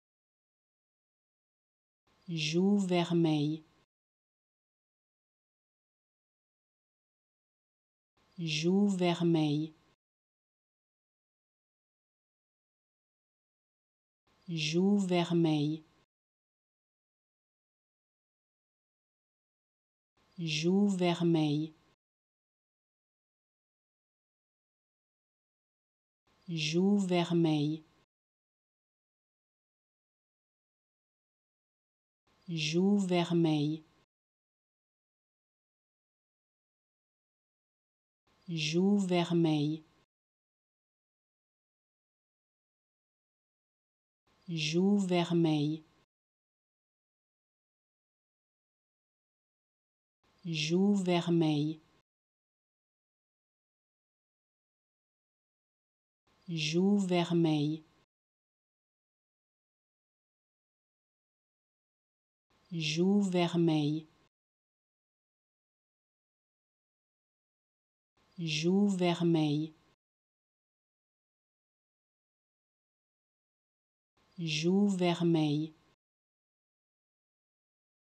How to Pronounce ''Joues vermeilles'' (Rime cheeks) Correctly in French
Learn how to say and properly pronounce ''Joues vermeilles'' in French with this free pronunciation tutorial.